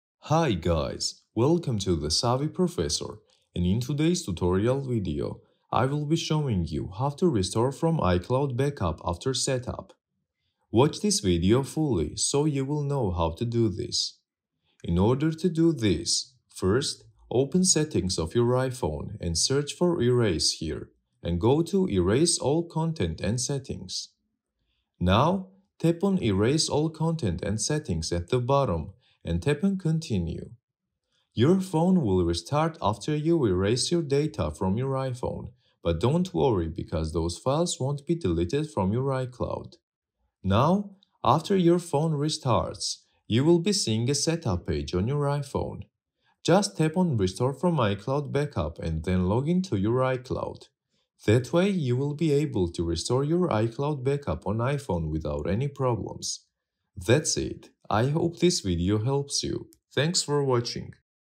How To Restore iCloud Backup After Setup (How To Recover iCloud Backup After Setup)
How To Restore iCloud Backup After Setup (How To Recover iCloud Backup After Setup). In this video tutorial I will show you how to restore iCloud backup after setup.